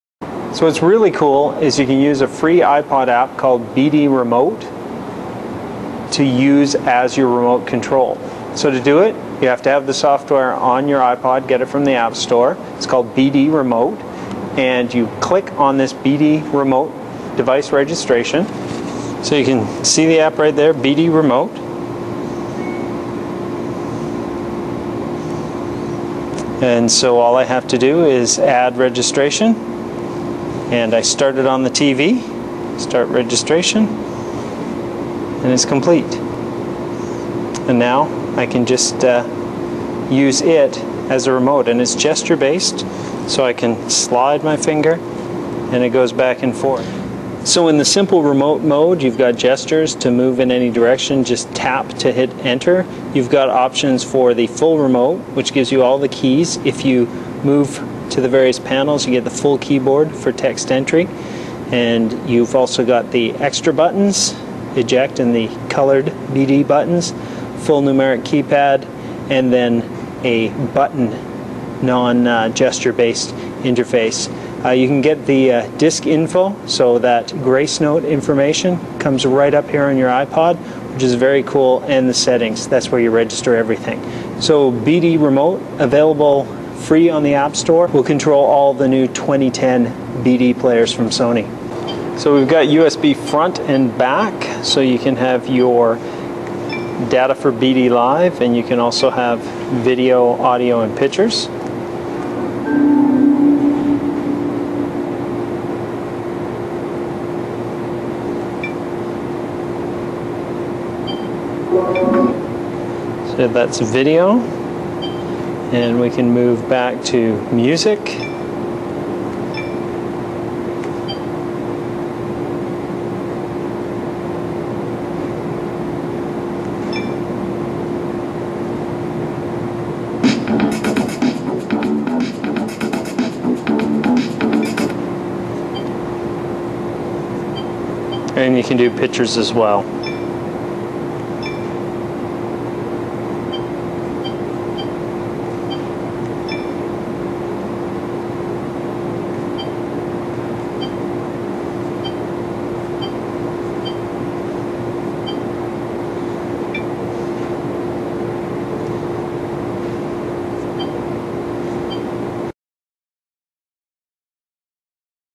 Sony Blu-ray Player BDP-S570 iPod app BD Remote demo Yoùtùbe - HD Technòlògy - Yoùtùbe - HD
The new Sony BDP-S570 Blu-ray player has 1GB of built in memory, built in Wifi, front and back USB, is 3D ready, and has a cool iPod app that allows you to u... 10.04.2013 Technòlògy - Yoùtùbe - HD